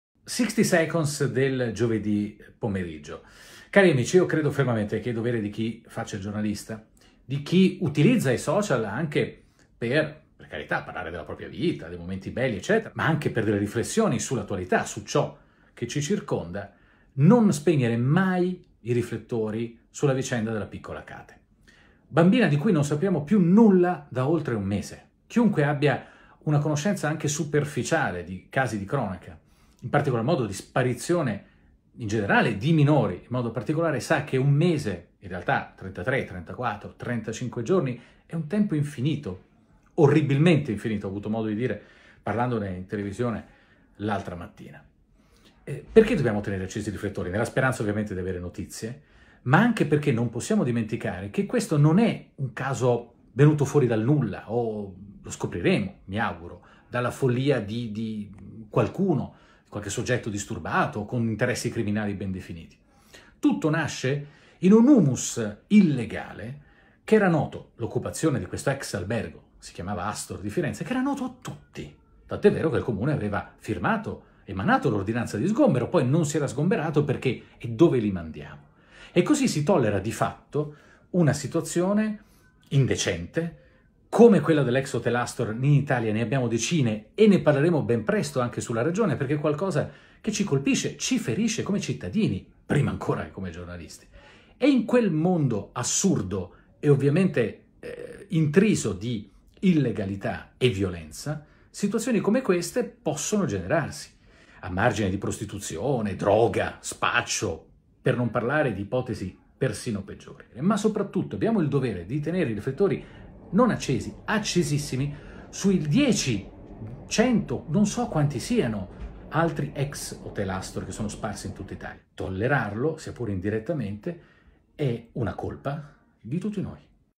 L’intollerabile silenzio sulle occupazioni abusive
60seconds del giovedì. Il dovere di tenere accesi i riflettori per la piccola Kata. E su quelle intollerabili cittadelle dell’illegalità che sono gli edifici occupati. Realtà conosciute e indirettamente tollerate da tutti. Il commento del Direttore Fulvio Giuliani.